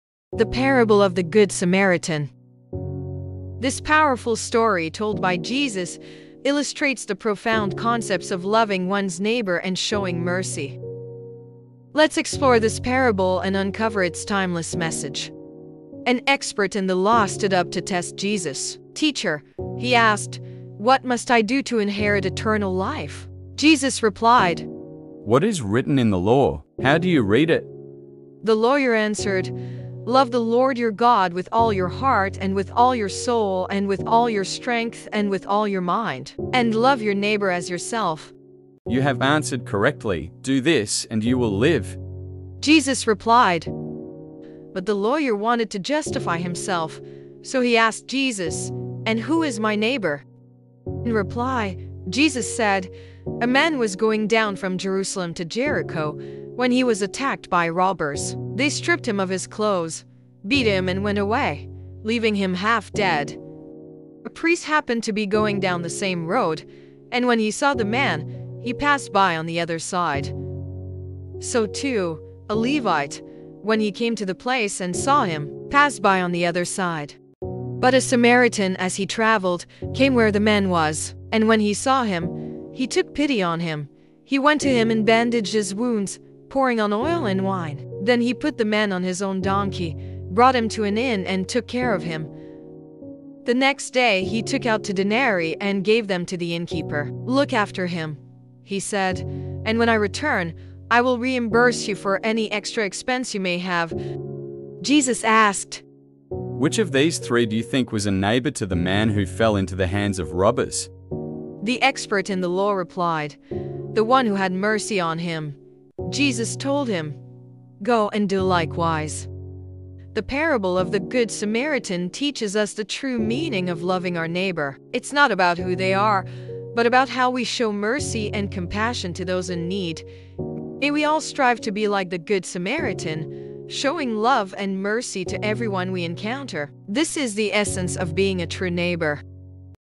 The Good Samaritan
The Good Samaritan is about showing love and mercy to everyone we encounter. This is the essence of being a true neighbor."

We create videos kids love, with values parents love.

Let's spread the message of faith, love, and miracles together!